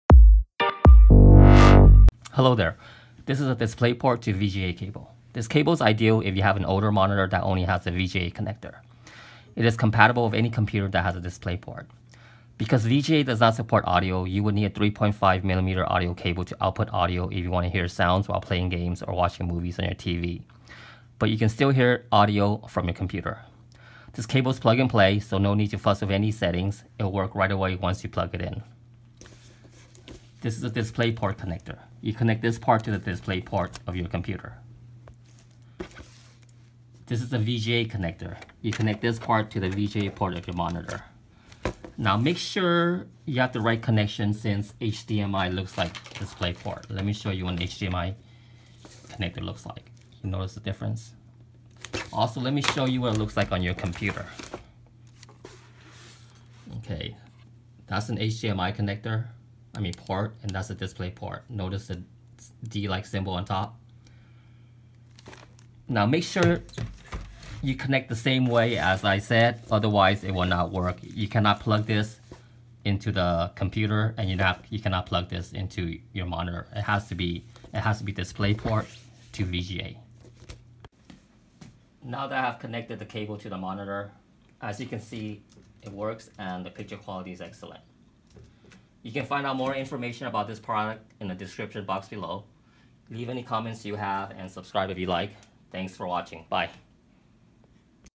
Connect your computer to monitor using Displayport to VGA Cable - Work on a bigger screen
Description: Displayport to VGA Cable
Part#: 10DP-DPVGA-06

Summary:

This is a Displayport to VGA Cable. This cable is ideal if you have an older monitor that only has a VGA connector. It is compatible with any computer that has a Displayport.
Because VGA does not support audio, you need a 3.5mm audio cable to output audio if you want to hear sounds while playing games or watching movies on your TV but you can get hear audio from
your computer. The cable is plug and play so no need to fuss with any settings. It'll work right away once it's plugged in.

This is a Displayport connector, connect this to the Displayport of your computer.
This is an VGA connector, connect this to the VGA port of your monitor, projector or TV.

Make sure it's the right connection since Displayport looks like HDMI.

This cable will not work if you reversed the connection like plugging the DVI part to your computer and Displayport to your monitor.